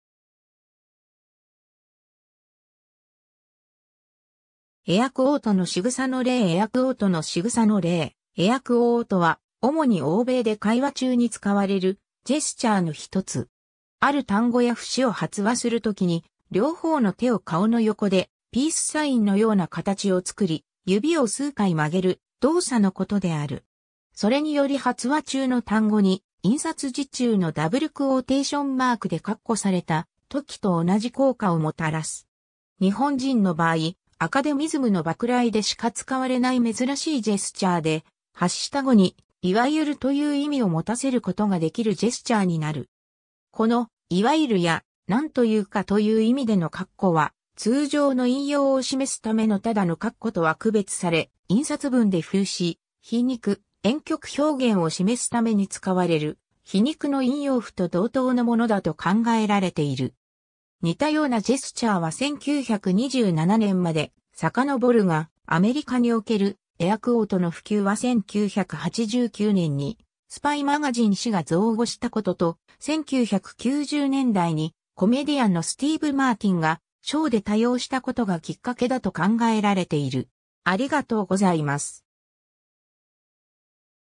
エアクオート
エアクオートのしぐさの例 エアクオートのしぐさの例 エアクオート（英: air quotes, finger quotes, ersatz quotes）は、主に欧米で会話中に使われるジェスチャーの一つ。
ある単語や節を発話するときに両方の手を顔の横でピースサイン（Vサイン）のような形をつくり、指を数回曲げる動作のことである。
それにより発話中の単語に、印刷字中のダブルクオーテーションマーク ("") で括弧されたときと同じ効果をもたらす。
 日本人の場合、アカデミズムの場くらいでしか使われない珍しいジェスチャーで、発した語に「いわゆる(所謂)○○」という意味を持たせることができるジェスチャーになる。
この「いわゆる」や「なんというか」という意味での括弧は通常の引用を示すためのただの括弧とは区別され、印刷文で風刺・皮肉・婉曲表現を示すために使われる皮肉の引用符 (scare quotes) と同等のものだと考えられている。
 似たようなジェスチャーは1927年まで遡るが、アメリカにおけるエアクオートの普及は1989年に"Spy Magazine"誌が造語したことと、1990年代にコメディアンのスティーヴ・マーティンがショーで多用したことがきっかけだと考えられている。